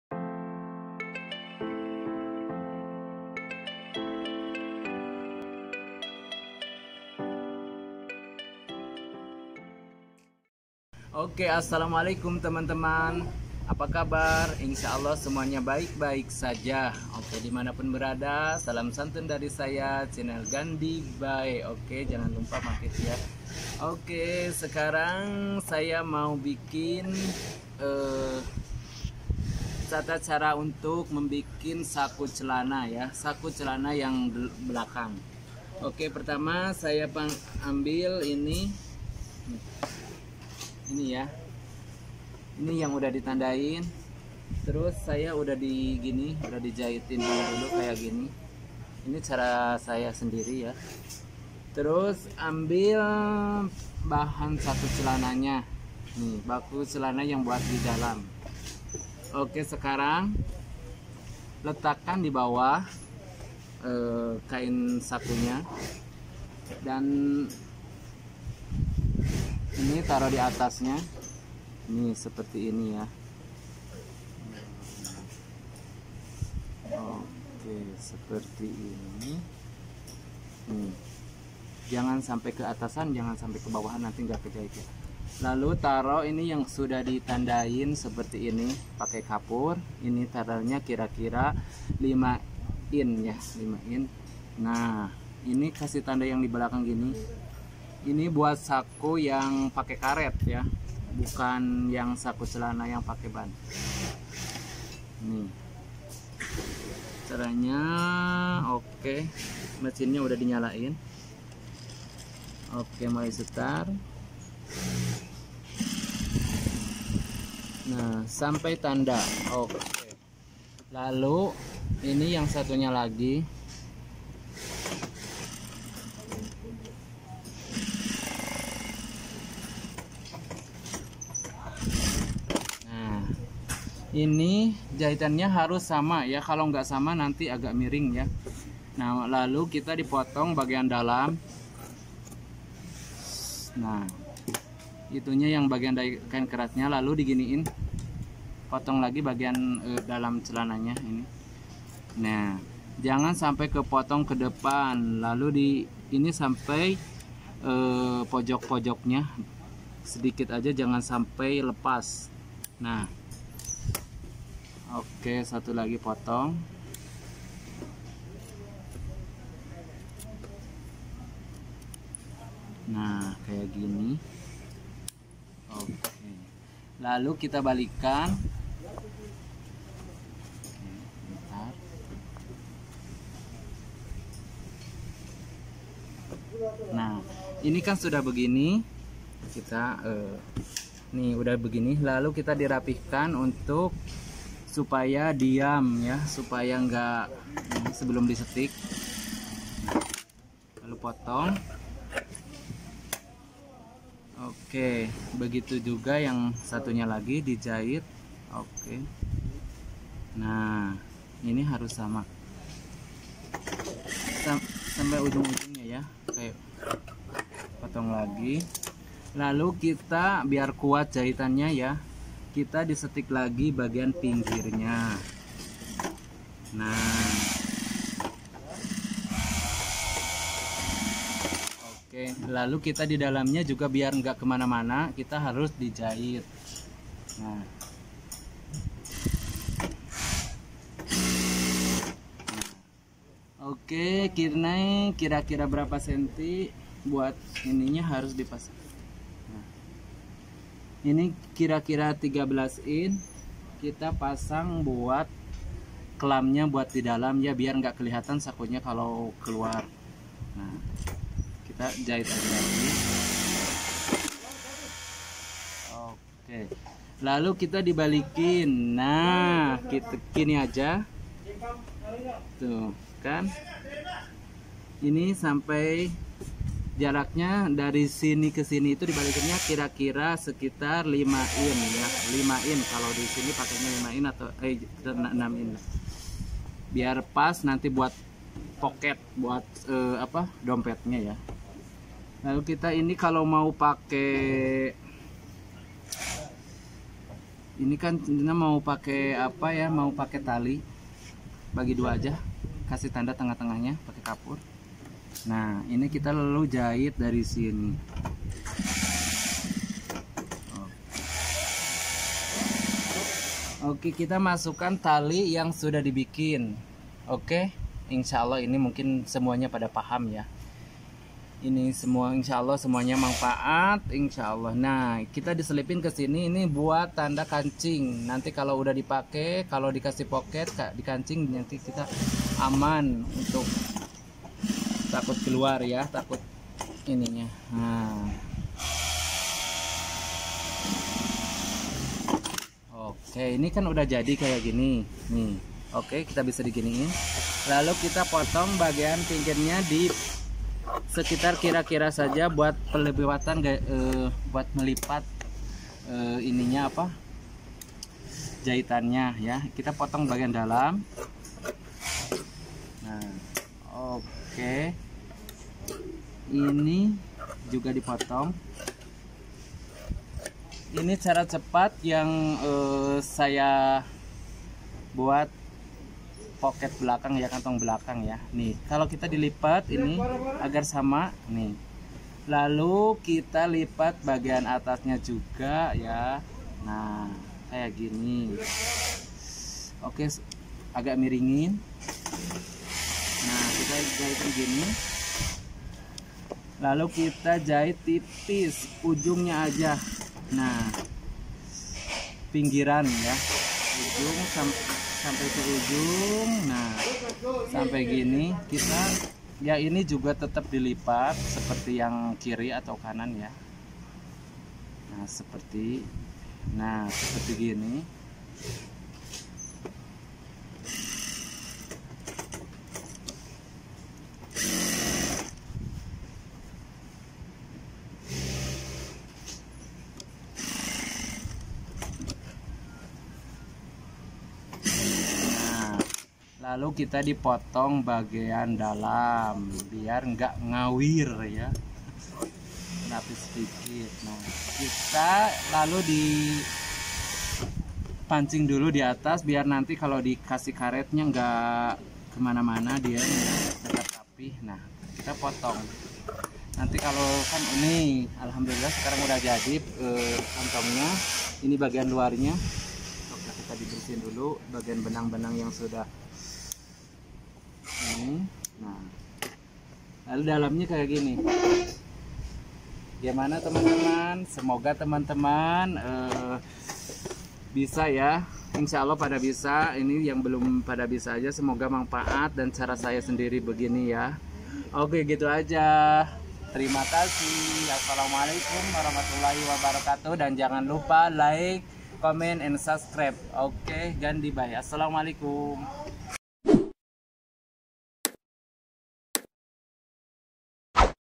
CARA MUDAH DAN CEPAT BIKIN SAKU BOBOK CELANA BELAKANG
Gandybae channel.mudah jahitnya gak ribet bikin saku celana belakang. Bisa cepat Tampa membuang waktu lama. Sangat mudah kan.